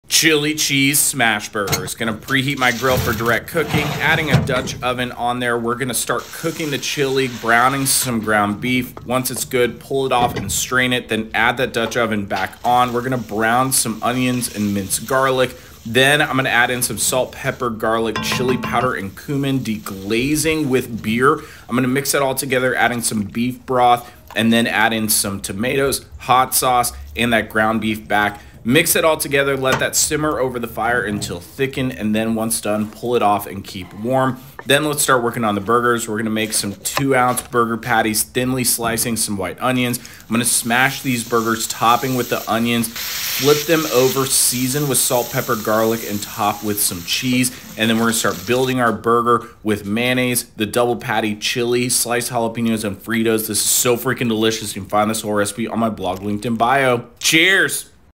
Chili Cheese Smash Burger Recipe | Over The Fire Cooking by Derek Wolf
Chili Cheese Smash Burger’s are messy but delicious! 🍔🌶🧀🔥

Everyone loves a good burger, but some people like to take it to an extreme. This Chili Cheese Smash Burger is the extreme. Smoky and meaty chili cooked over the flames topped onto a cheesy smash patty with onions. Served with some jalapeños and fritos for a nice crunch. 👍 🍻